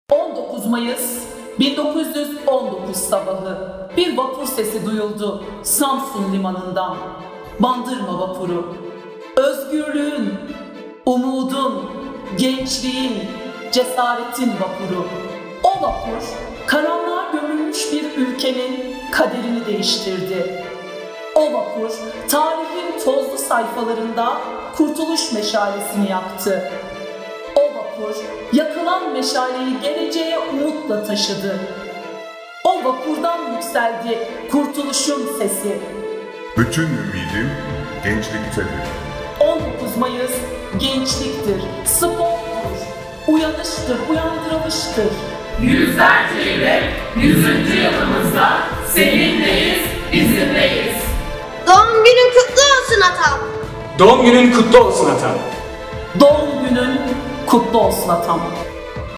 19 Mayıs'ın 100. Yılı Kutlu Olsun
1919'da Samsun'a çıkışının 100. yılında Atatürk'ü saygıyla anıyoruz. Gençlik ve Spor Bayramı'nı kutluyoruz.